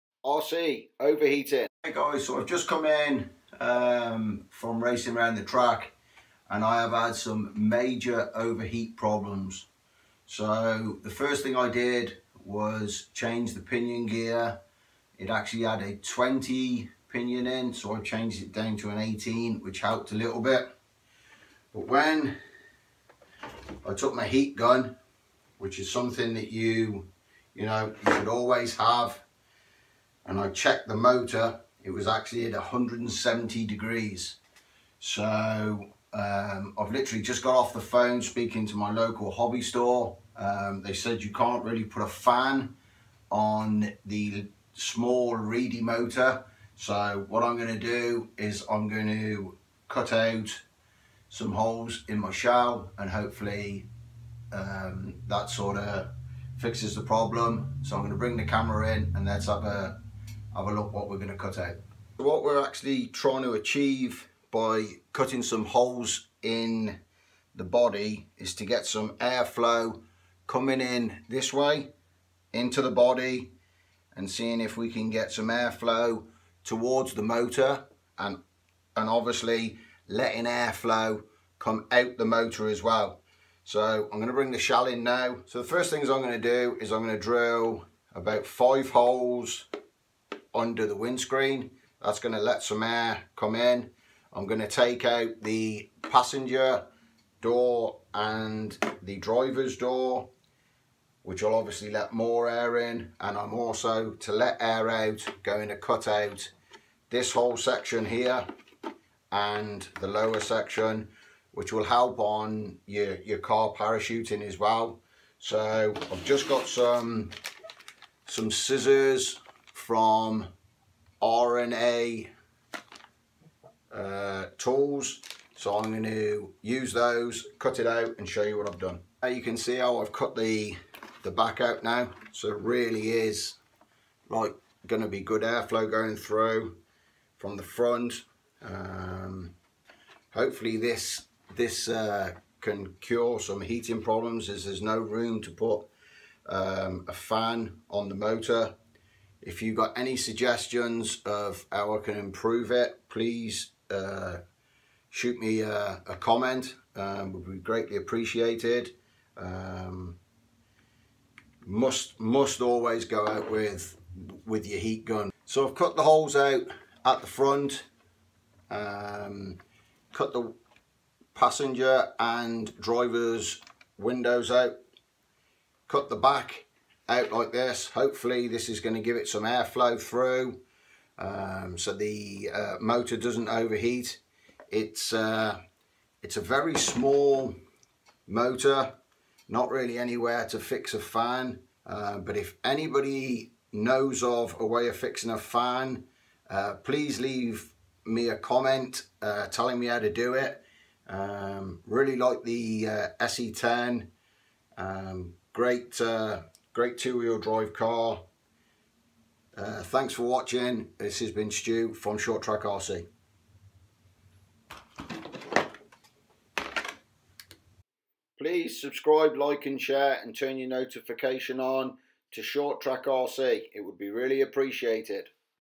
RC OVERHEATING- NEW BODY HOLES NEEDED!!!!!
had trouble @ short track rc when my rc motor hit 170 degrees hot ,hot, hot
so i brought my sc10 short course truck in and changed the pinion to a 18 from a 20 and also put some holes in the body and cut out the side windows and some parts out the back of the truck